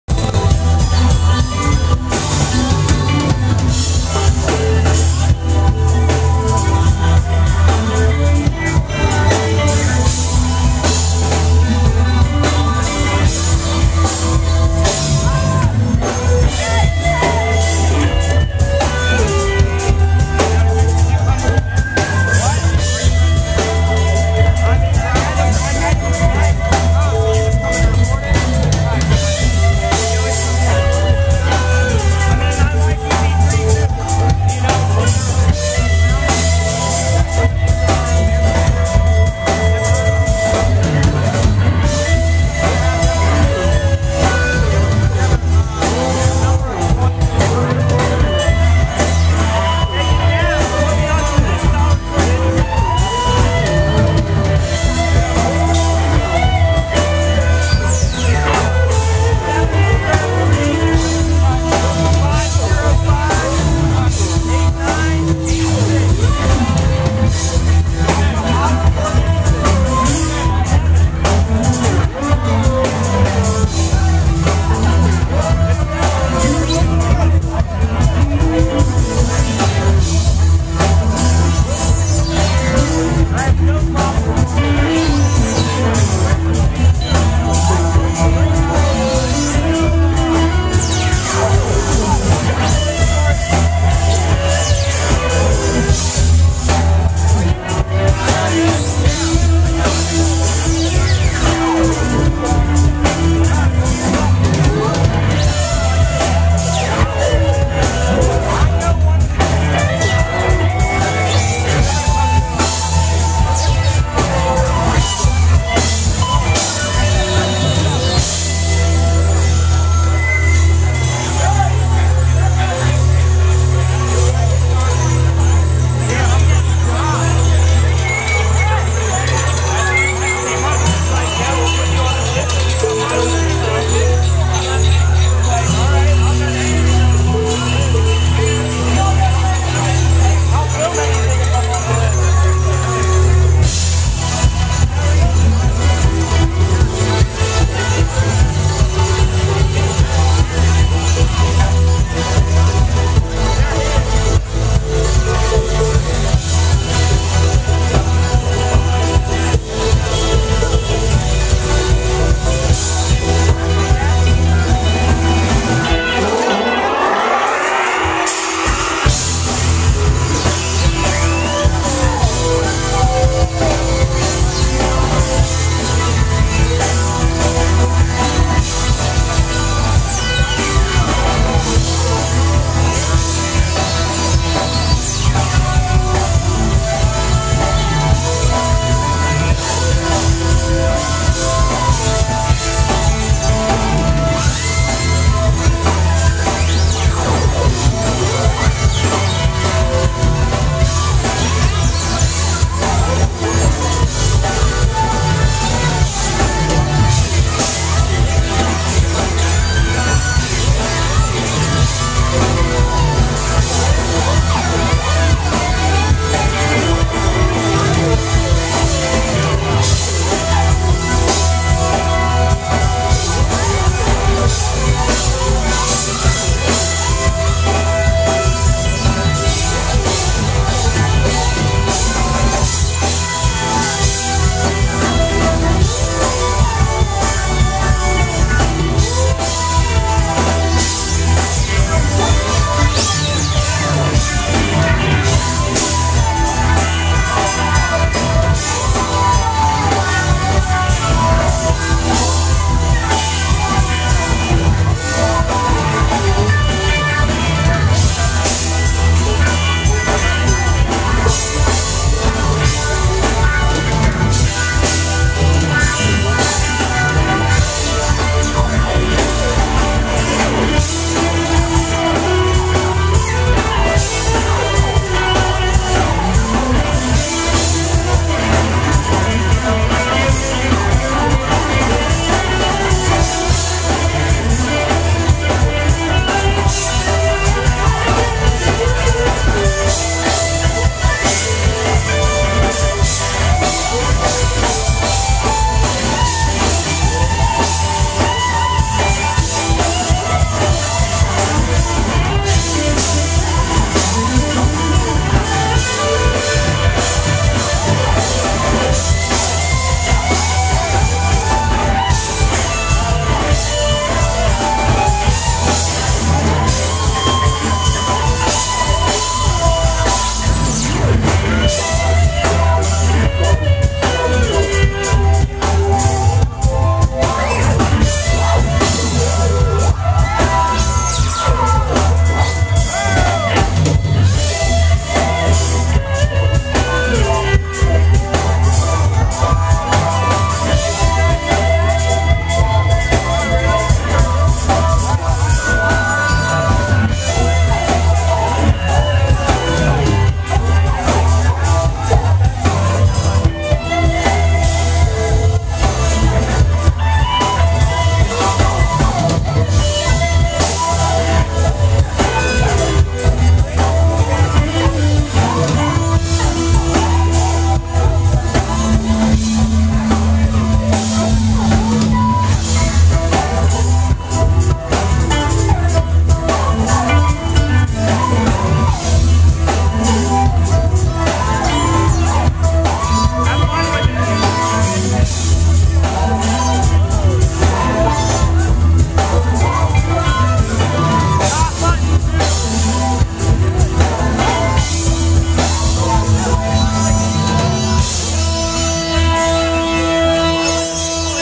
EP3 @ Crowbar 6/4/2010
EPIC ELECTRO ROCK! what a night @ Crowbar w/ EP3 the Produkt and DJ Craig Heneveld!